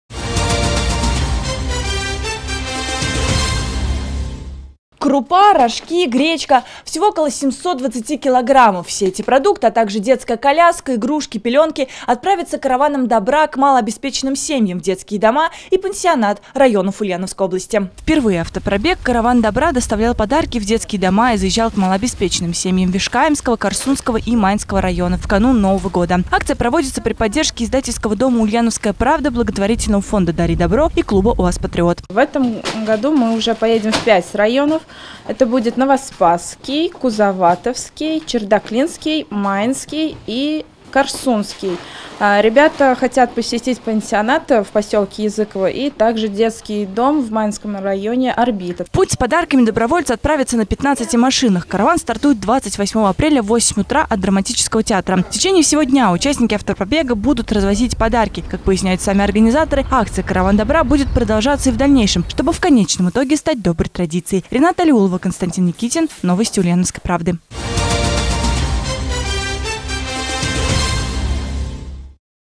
«Караван добра» на УАЗах отправится в пять районов Ульяновской области
Благотворительная акция стартует во второй раз.